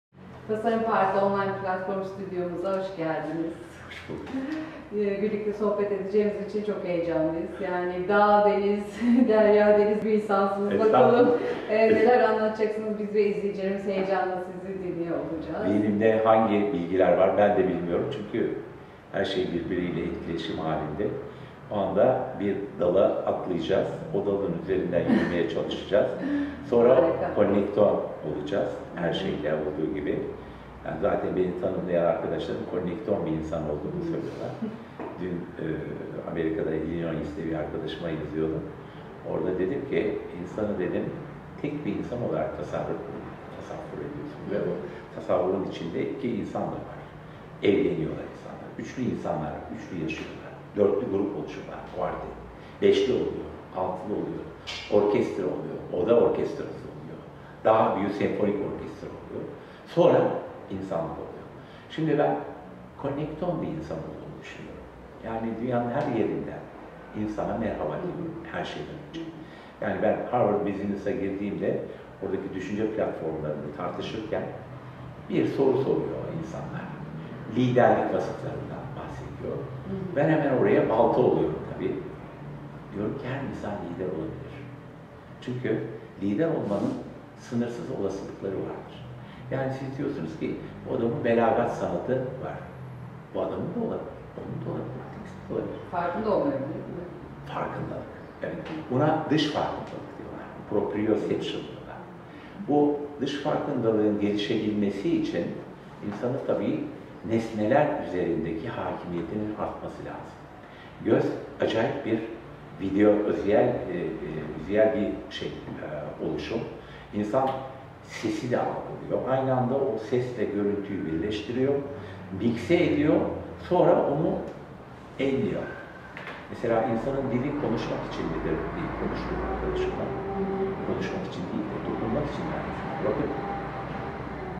Araştırmacı Yazar Kemal Çağın ile söyleşi
Tasarım Parkı Online Stüdyo'nun bu haftaki konuğu renkli kişiliği ve entellektüel kimliği ile Araştırmacı Yazar Kemal Çağın.
Kemal Çağın ile bugünün dünyasına, tasarım, kuantum mekaniği boyutundan baktığımız röportajı keyifle izlemeniz dileğiyle... Kemal bey ile röportaj serimizin devamı çok yakında Tasarım Parkı kanallarında yer almaya devam edecek.

Kemal Çağın
İstanbul'da doğdu. 1976 yılından bu yana yayın sektöründe görevli. Bitkievi’nin yöneticisi. Bitkiler konusunda araştırmalar yapıyor. Tema Eğitmeni olarak bugüne kadar 200’ün üstünde konferans verdi. Fuji Film’le birlikte 40’ı aşkın çevre sergisi açtı. Paris’te Şifalı Bitkiler üzerine Jardin des Plantes’da incelemeler yaptı ve Flora ile ilgili 30'u aşkın sergi açtı. DHKD, IFSAK, ÇYDD üyesi, Tema gönüllü eğitmeni.

Halen bitki araştırmacısı ve ressamı olarak Bitkievi’nde çalışmalarını sürdüren Çağın, Avrupa ve Türkiye florasını resimleyerek, 25.000 bitkiyi kapsayacak bir katalog çalışmasını yürütüyor. Bu alanda yazarın “Avrupa ve Türkiye Florası İnceleme Dizisi” adı altında çıkmış yedi kitabı bulunmaktadır.

Ayrıca Çağın, aylık Buğday, Housebeautiful, Doğa ve Sağlık ve Gezgin dergilerinde araştırma yazıları yayınlamakta ve flora resimleri çizmekte. Geo Dergisi’ne bitki danışmanlığı yapmakta, NTV’nin hazırladığı İstanbul Ansiklopedisi'nin "İstanbul Bitkileri" ile ilgili metinlerini hazırlamaktadır. Bitkievi’nde ise 5 yıldır “Bitki Seminerleri” vermekte, öğrenci yetiştirmekte ve Anadolu Florası'nın tohumlarını toplamaktadır.